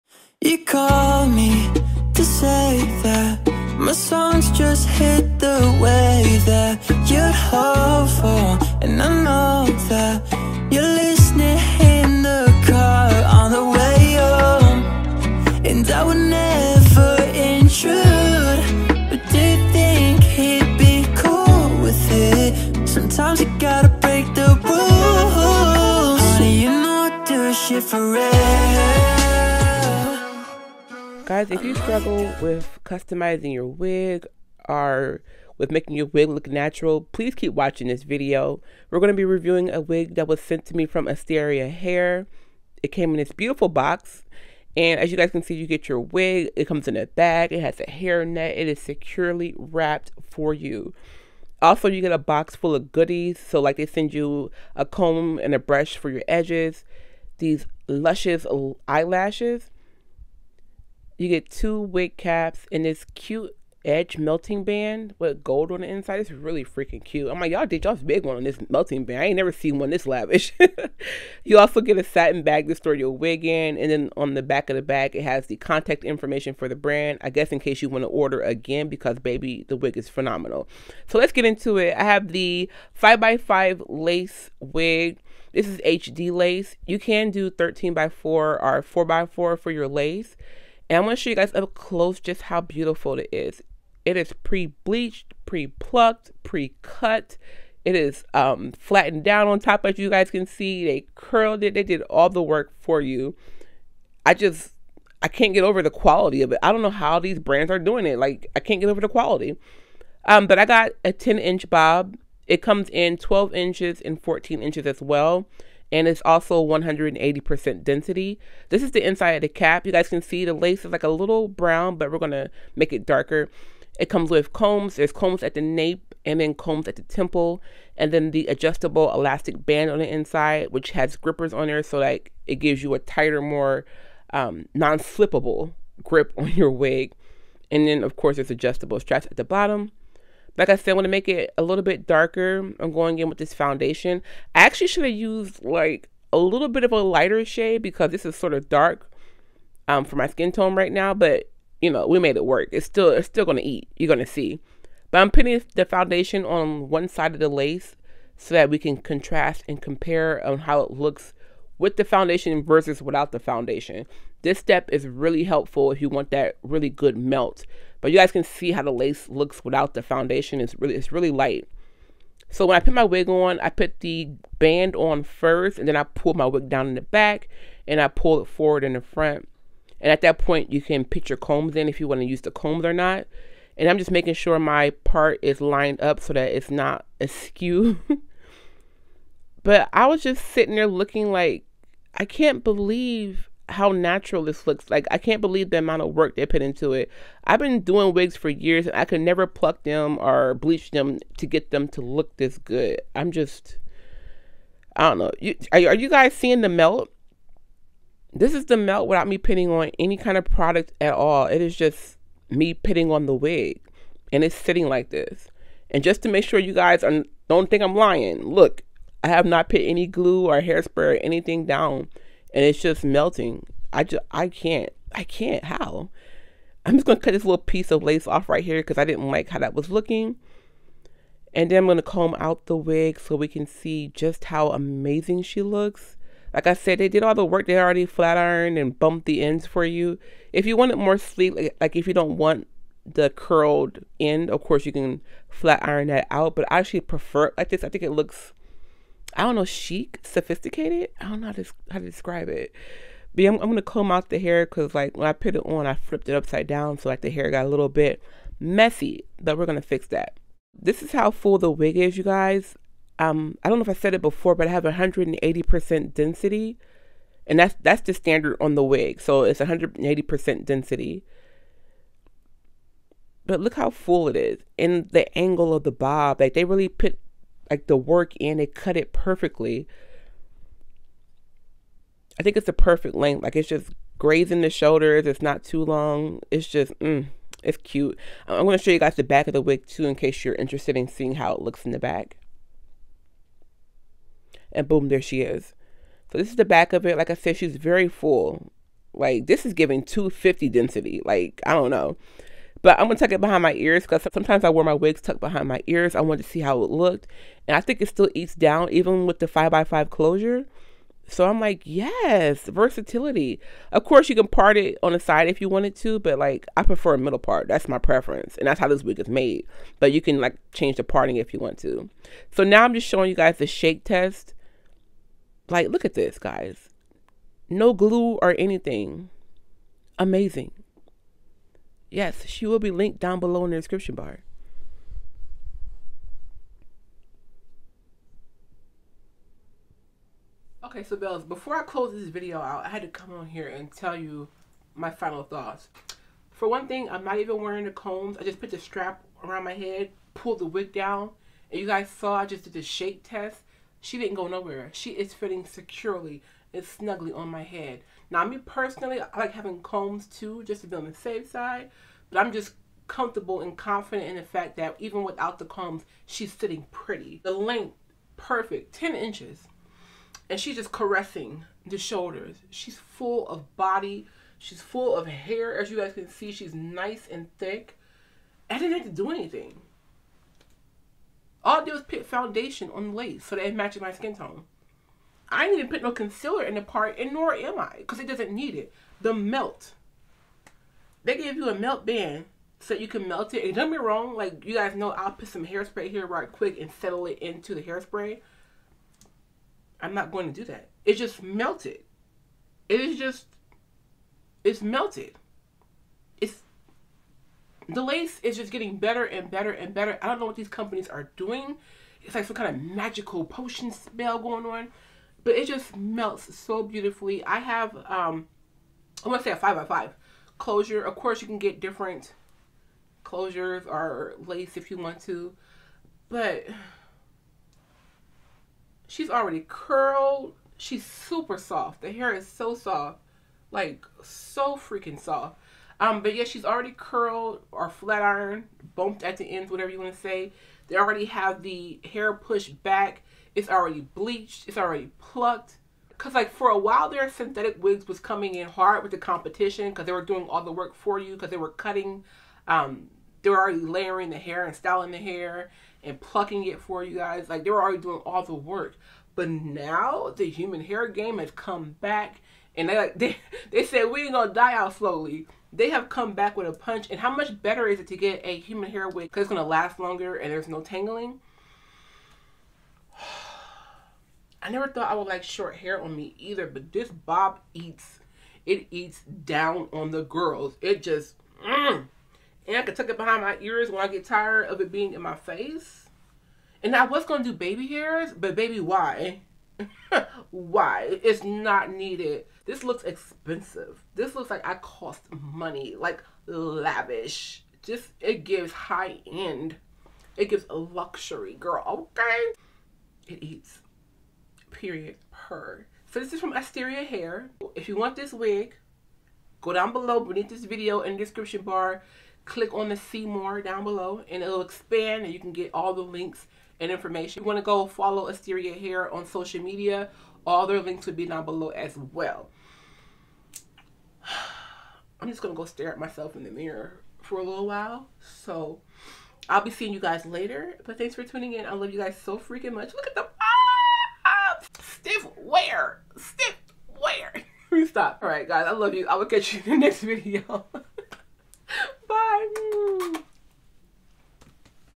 Straight Out The Box This Grip Wig Is Perfect! Watch This Instant Melt With No Glue! | Asteria Hair
Hair Info: 10 inch pre cut straight bob HD 5x5 lace closure wig

⬇️   Connect With Melissa ⬇️
 I N S T A G R A M➜ @ iluvMelissaDanielle
T I K T O K➜ @iluvmelissadanielle